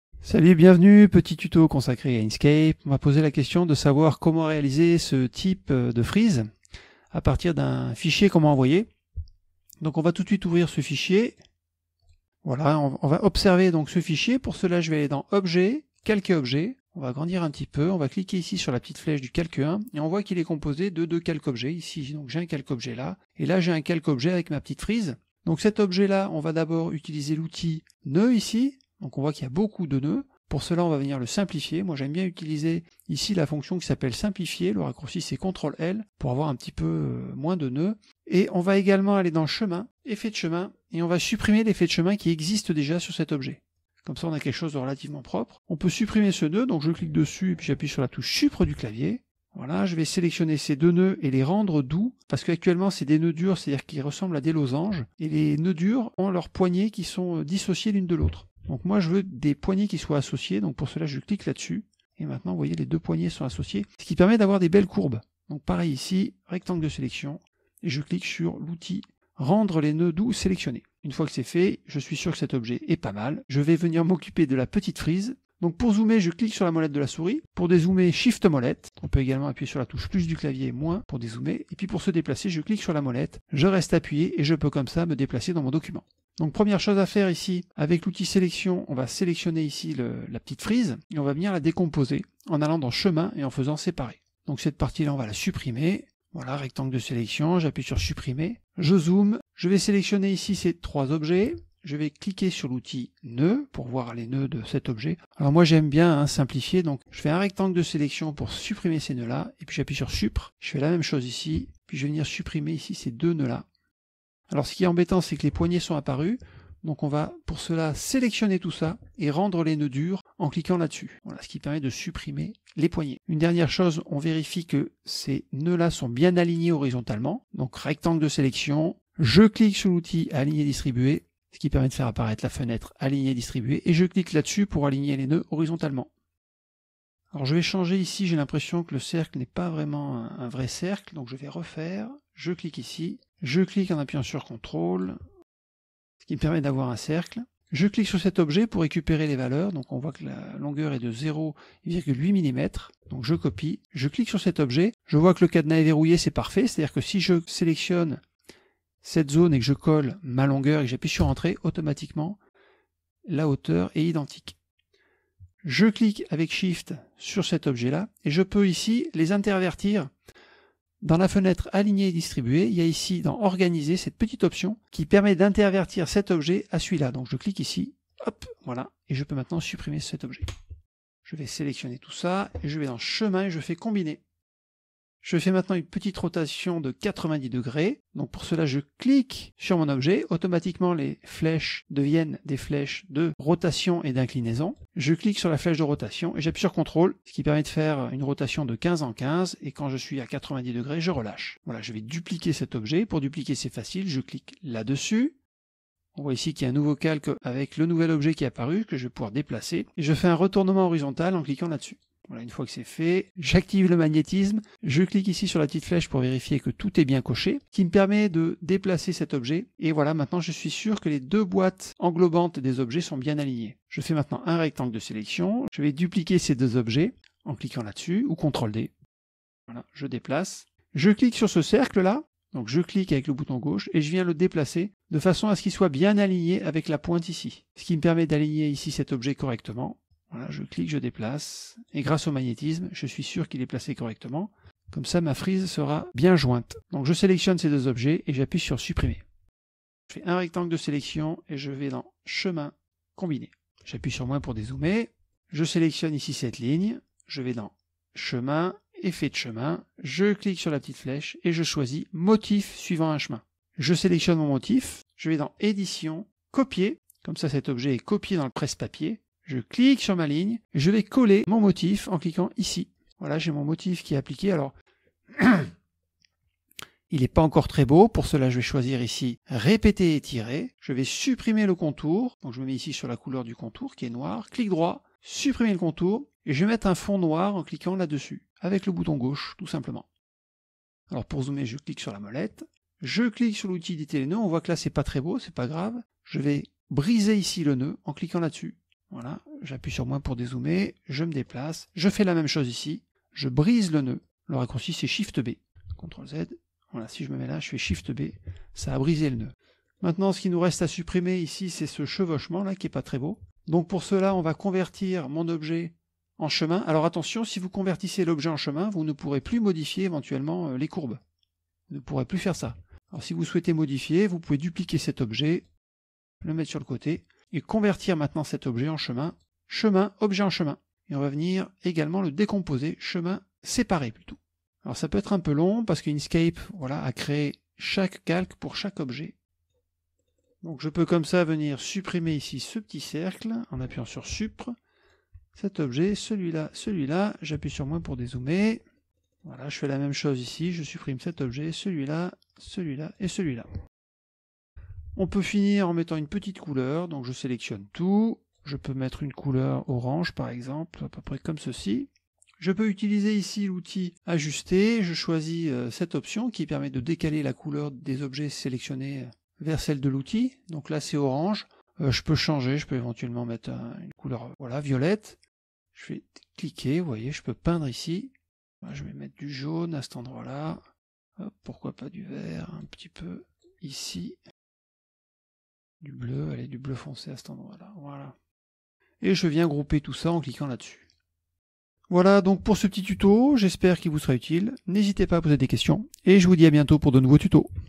Tuto inkscape : faire une frise (question/réponse)
Nous allons voir comment faire/créer/modifier une frise avec inkscape 1.4